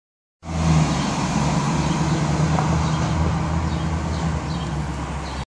WS6 Pontiac Trans Am Acceleration
Defunct brand Pontiac made stand out vehicles. Here's a prime example, an 2002 WS6 Pontiac Trans Am accelerating! Share on Pinterest, Tumblr, forums, blogs.